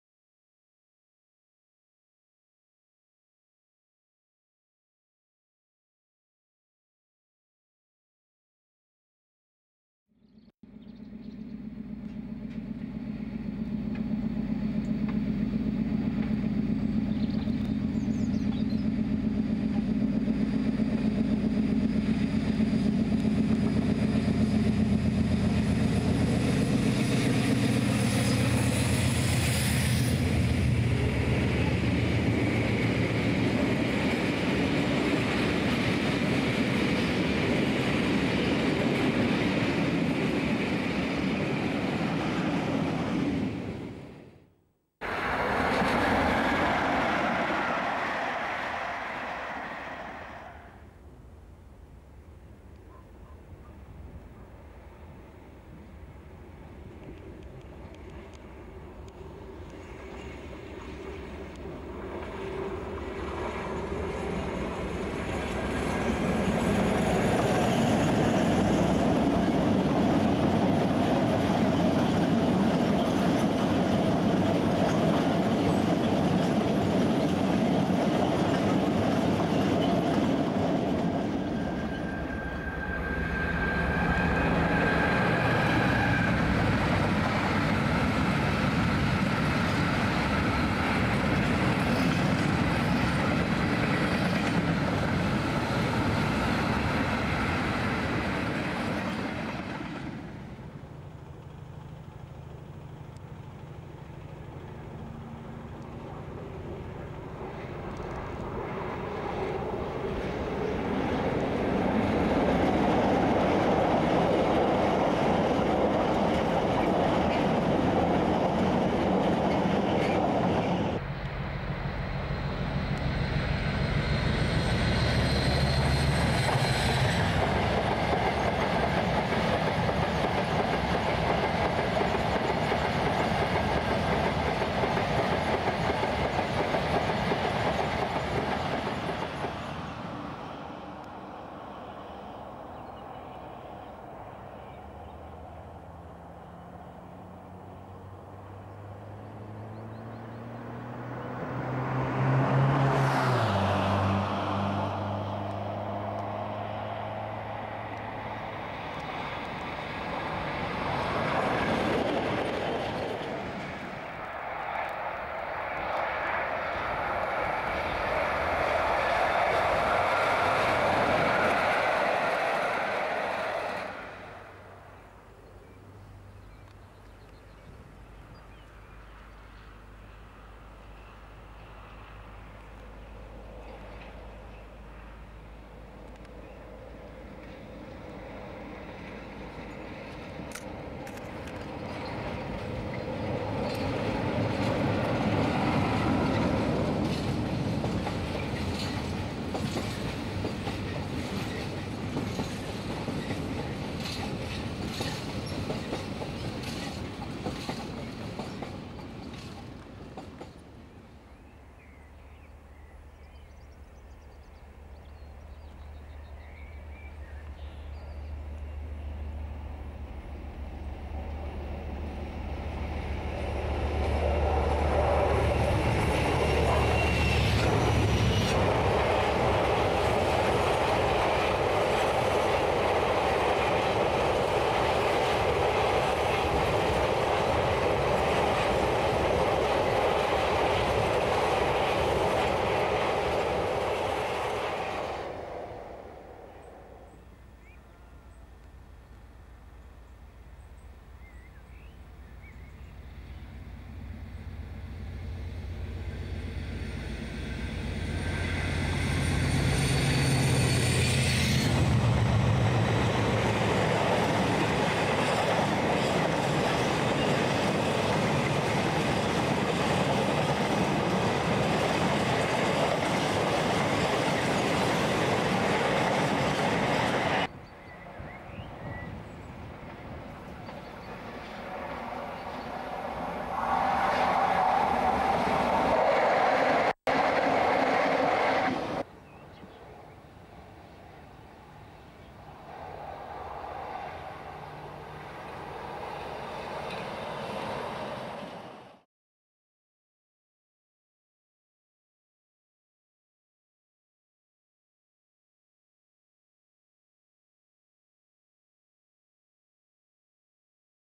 Railways. British Railways South wales mainline misc 1996 No2 47s and 56s (mostly)
5 minutes of clips from the South Wales mainline Coedkernew , Dyffryn Newport Miskin etc showing everyday comings and goings over a period in 1996 featuring mainly class 47s and 56s.
the first clip I like is 56033 which I believe was working the returning Aberthaw oil bogie tanks but making hard work of it .